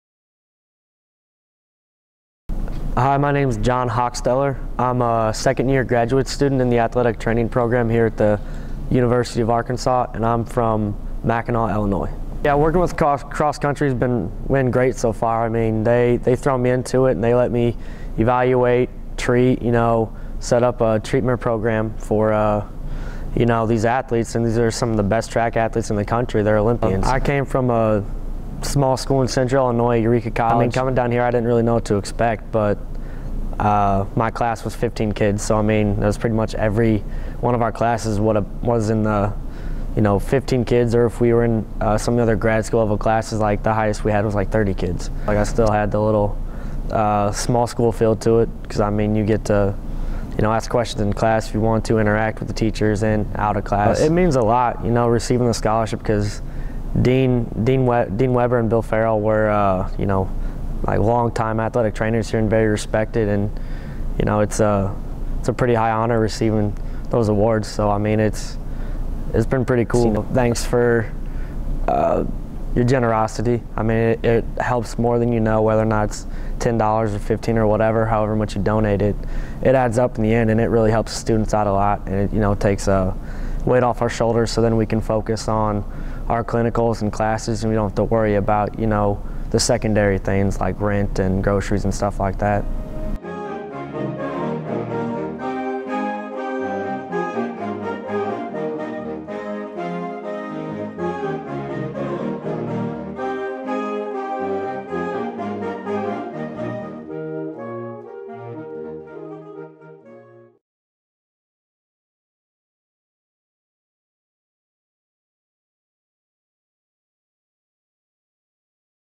Graduate Student Jon Hochstetler Thanks Annual Fund Donors
Especially during this time of year rich in giving and receiving, we want to thank you for supporting the University of Arkansas. Your generosity is appreciated and helps more than you know. Gifts of every amount add up and really make a difference for students like Jon.

This video and its contents are a product of the Office of Annual Programs at the University of Arkansas.